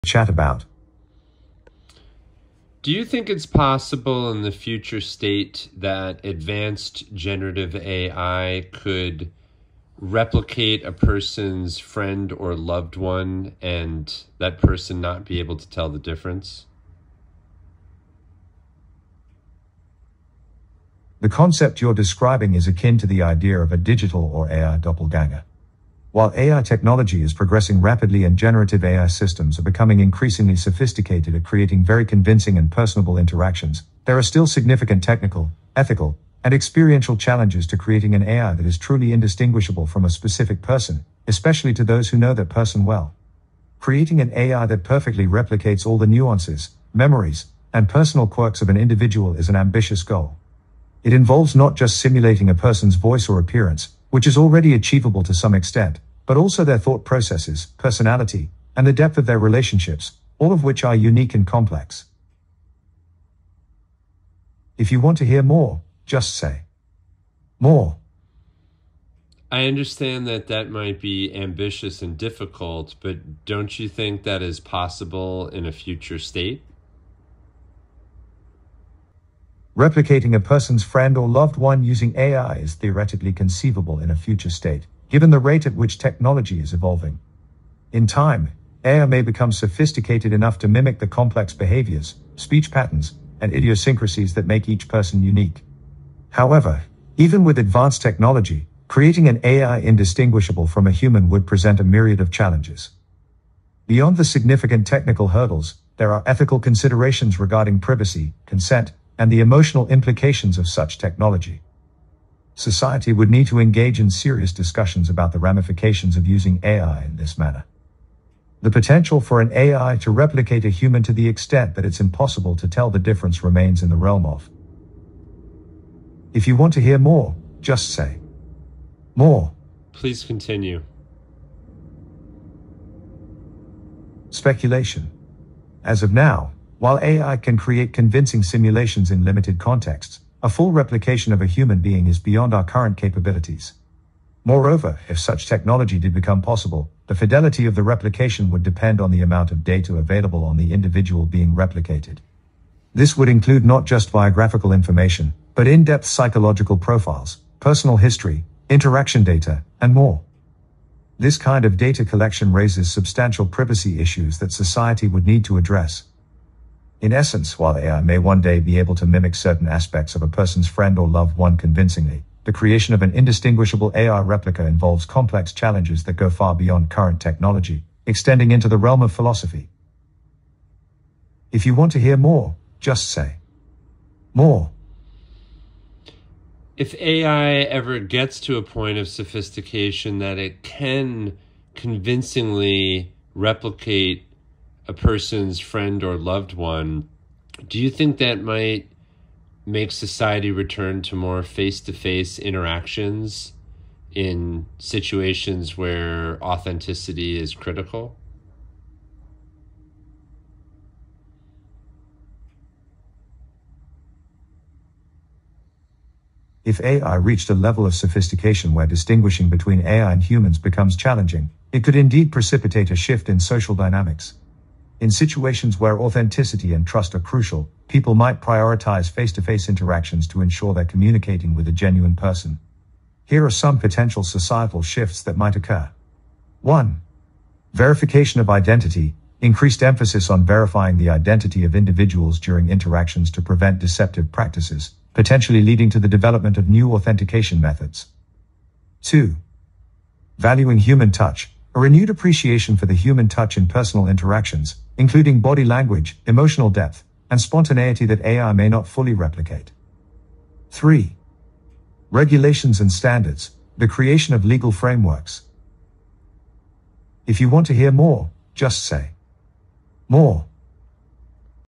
Talking to AI about the dangers of AI
I do legitimately fear a future where we will no longer be able to trust visual or audio identities, and the deep harm that that could cause society. But my conversation with chat GPT about this, made me feel a lot better about it. 😀